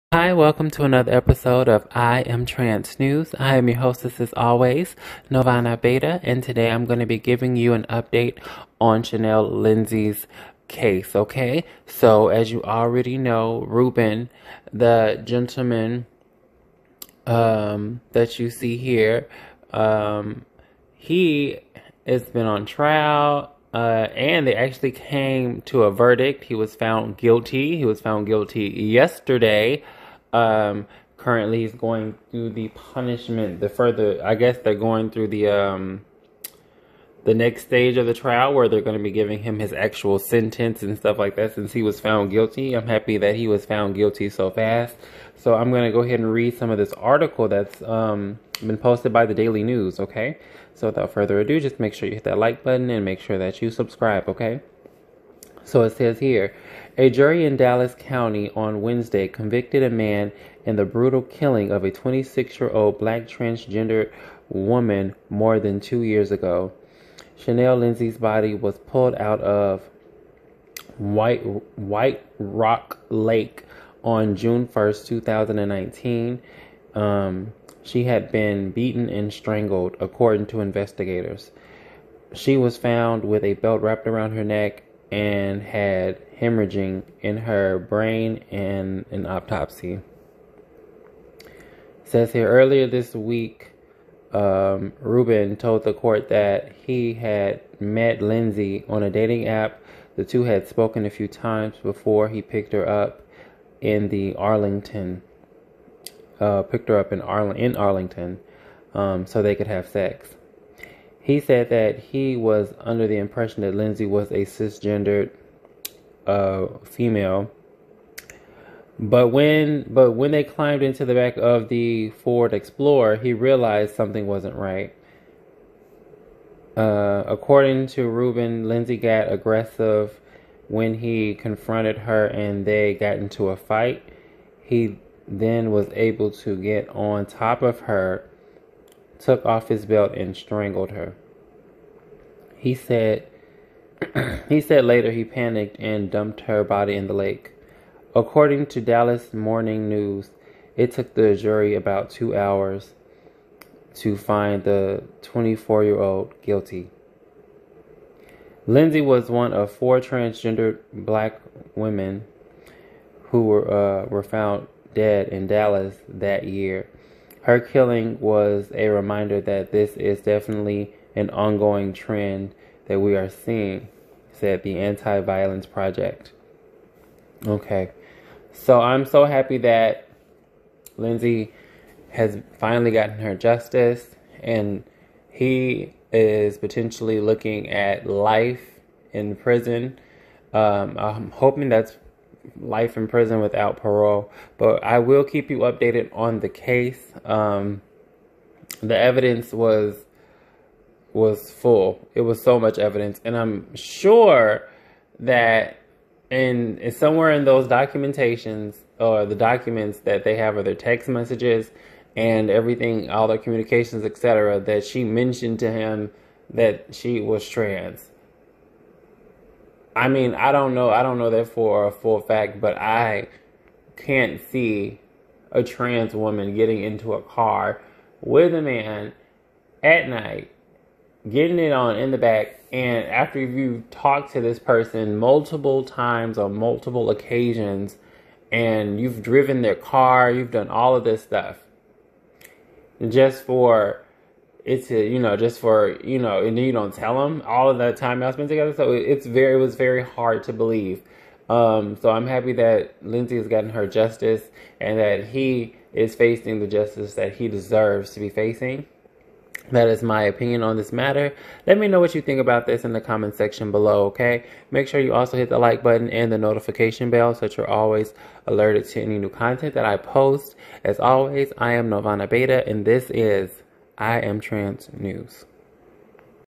Jury Took 2hrs To find Ruben Alvarado Guilty
Ruben Alvarado claims he didn't know that Chynal Lindsey was transgender and he was afraid, so he hid her body after he killed her. It took the jury 2 hours to find him guilty and he faces life in prison